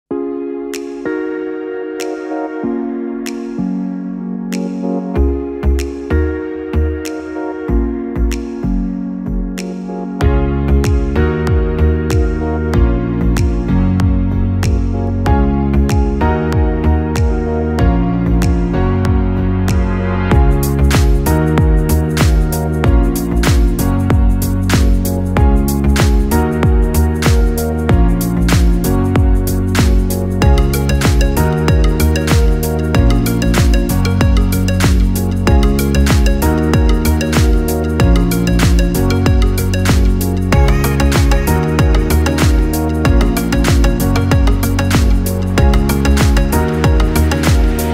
WSGI android app promo
Listen Christian Music, Podcast, and community events, Available in the Play Store.